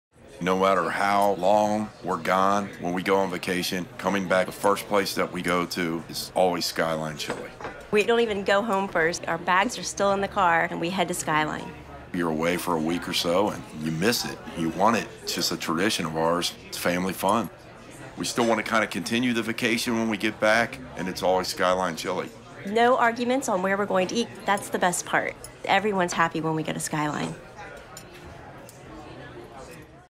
Rick & Family – First Place We Go
Brandience has launched a new campaign for Skyline Chili featuring real customers sharing authentic stories in their own words about their emotional connection to the brand.